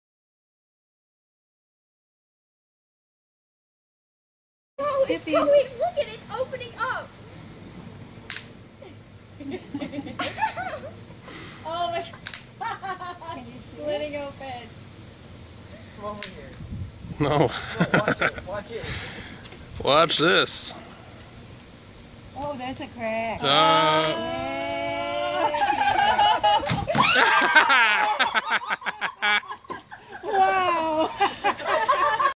Movie No 2 20140901 165130
How to explode a Watermelon with rubber bands.